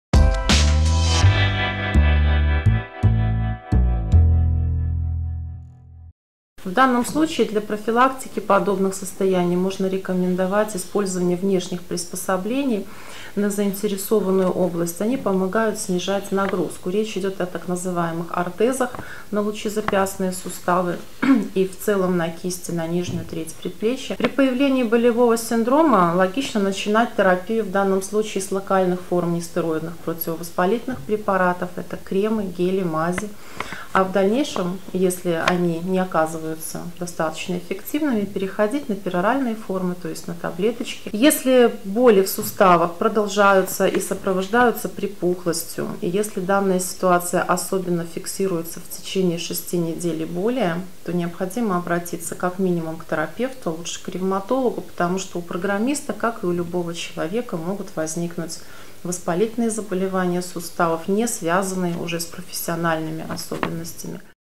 Что можно сделать для профилактики болезней суставов?
Консультация доктора в прямом эфире. Что можно сделать для профилактики болезней суставов? Узнайте за 1 минуту 4 секунды